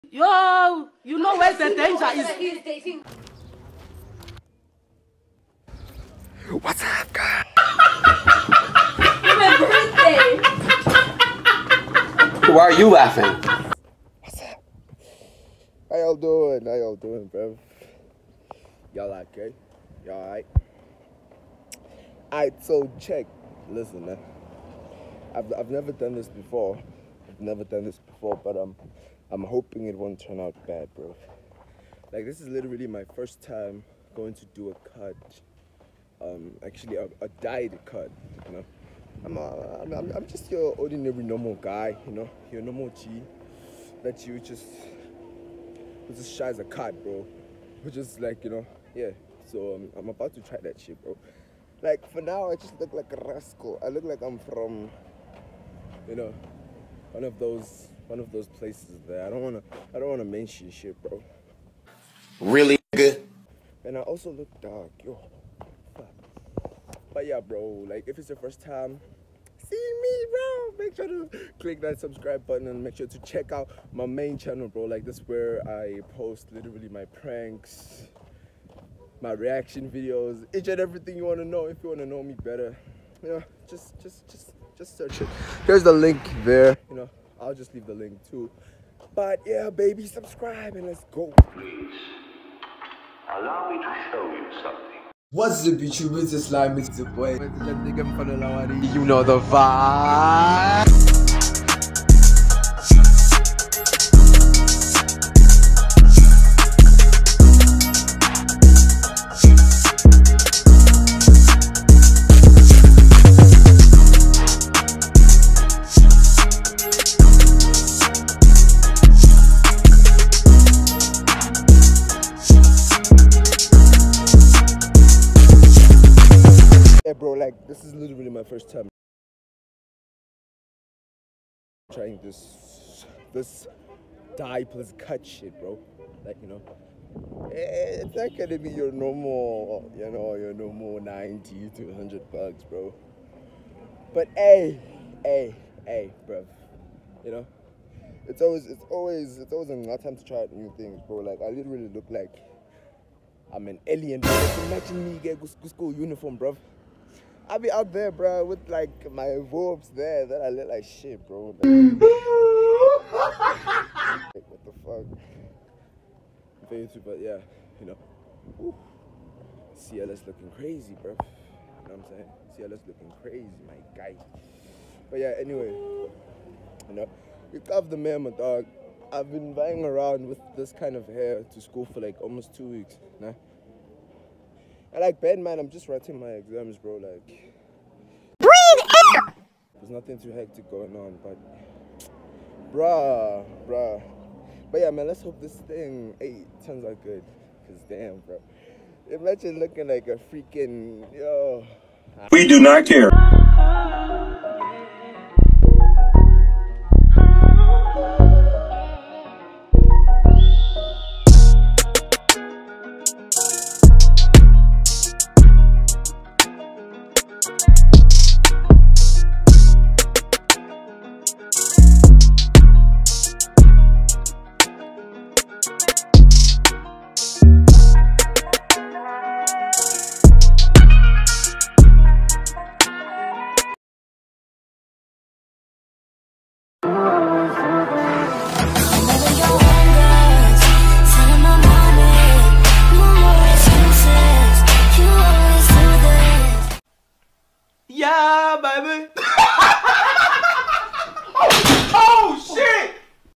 $LIMEVLOGS:FIRST TIME POLISHING MY HAIR😳🇿🇦
Heeeyy Wake Up Time to play Is Fucked up😂it’s  your slime it’s your boy it’s your gent kemfanola wadi You know the Vibes and I gotta say one thing I was supposed to upload this on the Vlog Channel but no subs🤷‍♂️🫣Ayikho Subscribers there😭😭But Anyway I hope y’all enjoy this 4:59 minute video🥺🔥I’m back and For good y’all get this vid to 5 likes i’ma upload a young prank😂🤟🏼 @Reofentse_yaslimevlogs